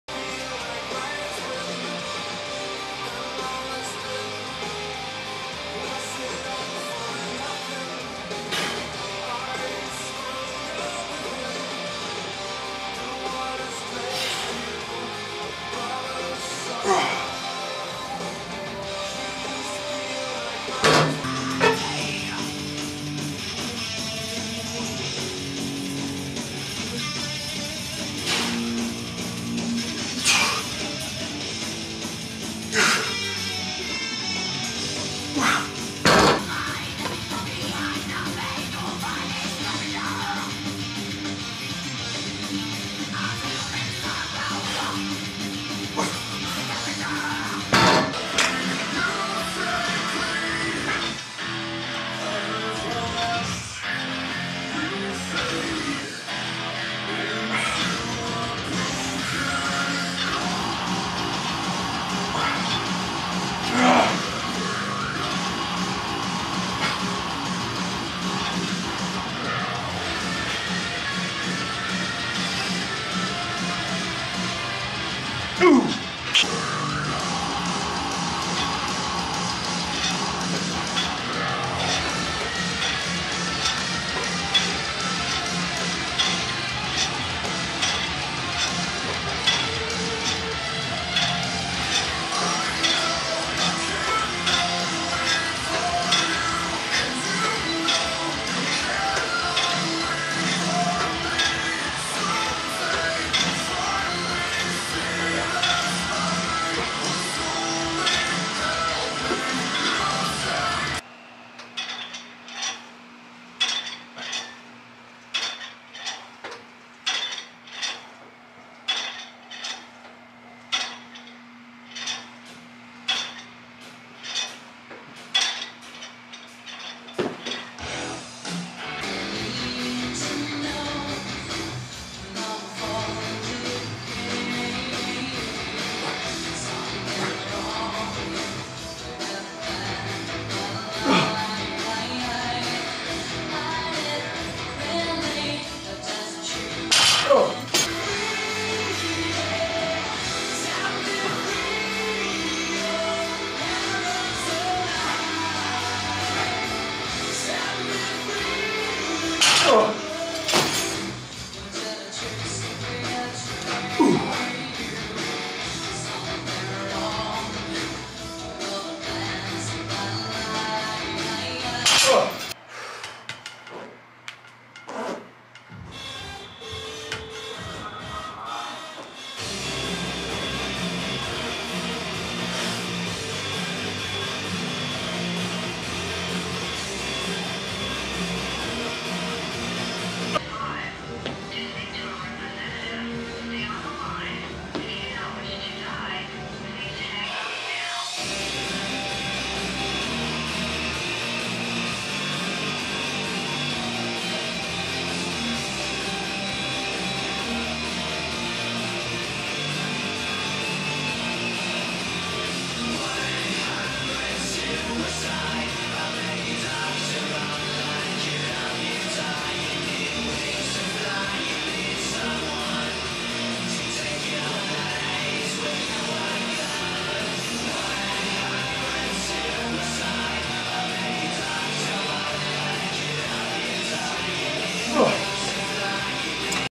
Men's Physique PULL workout (Promaxima T-Bar Row, Prime Fitness Attachments)
Solid workout tonight with some new equipment!  Featuring the Promaxima Chest Supported T-Bar Row and numerous attachments from Prime Fitness.